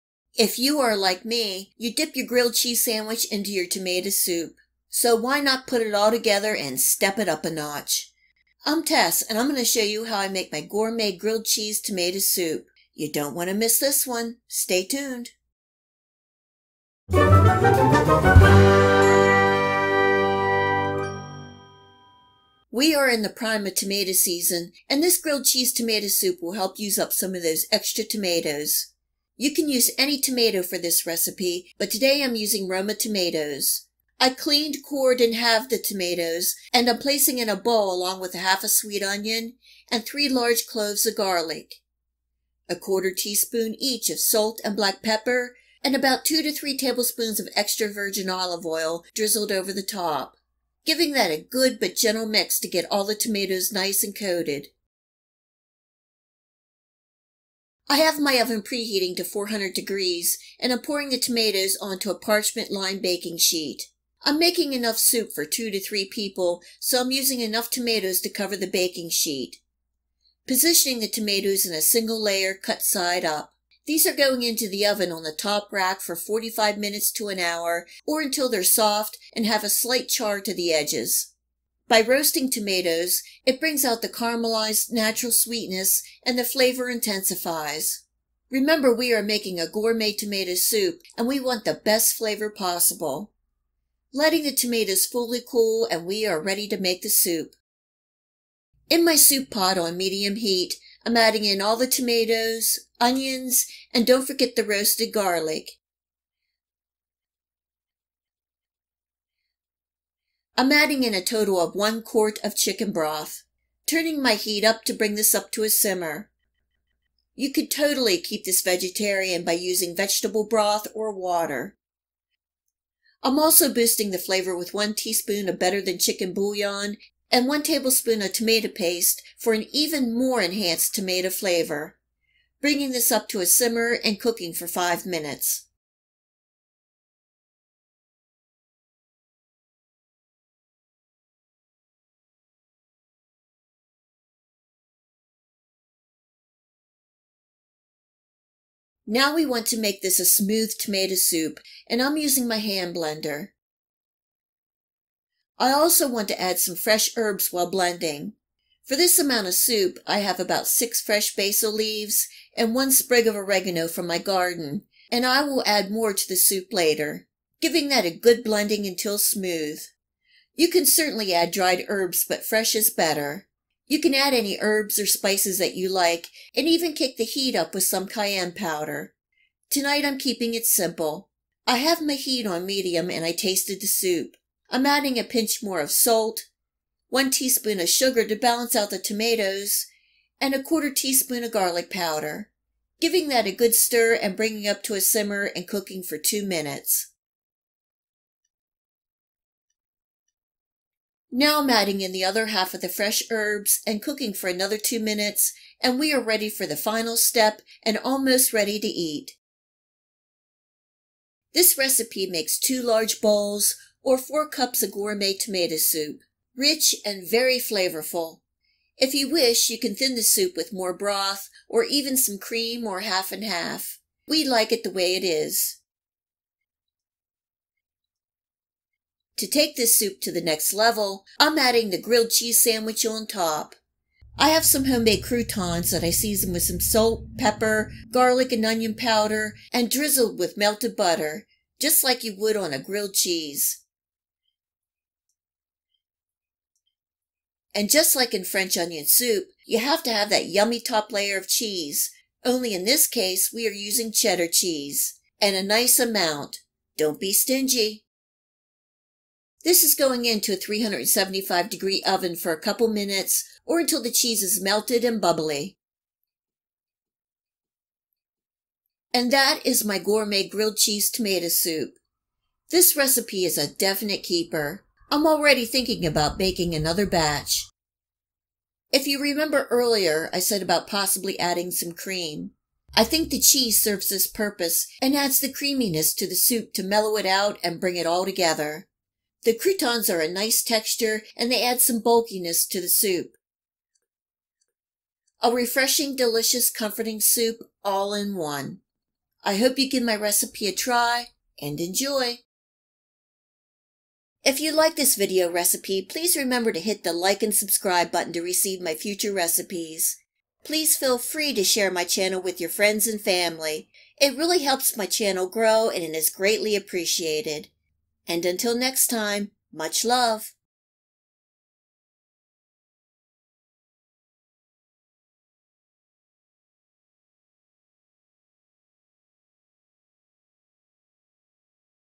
Gourmet Grilled Cheese Tomato Soup Recipe - All in One Bowl!
Homemade tomato soup from scratch with a TWIST of a grilled cheese sandwich on top. A great way to use those garden tomatoes. RECIPE BELOW! Enjoy!

INGREDIENTS:
3-4 lbs. Roma tomatoes - cleaned, halved and cored
1/2 medium onion
3-4 large cloves garlic
1/4 tsp. each salt/black pepper
2-3 Tbsp. olive oil
*Mix together in bowl

1 quart chicken broth
1/4 cup packed fresh basil leaves - divided
2-3 fresh sprigs oregano - divided
1/4 tsp. garlic powder
salt and pepper to taste
1 tsp. sugar
croutons

In a large bowl place tomatoes, onions, garlic, salt, black pepper and extra virgin olive oil.
Toss until coated.
Place on parchment lined baking sheet single layer cut side up.
Bake at 400 degrees for 45min to 1 hour until tomatoes are soft and slightly charred or edges.

Place tomatoes/onions/garlic in soup pot on medium heat.
Add broth, bouillon and tomato paste.
Bring up to simmer and cook for 5 minutes.

Add half of fresh herbs and blend until smooth.
Add seasonings and cook for 2 minutes.

Add second half of fresh herbs, stir and cook for another 2 minutes. Remove from heat.

Ladle tomato soup into oven proof soup bowls.
Top with croutons and a nice layer of cheddar cheese.

Bake in 375 degree oven for a minute or two or until cheese is melted and bubbly.

Tess Cooks4u Amazon Store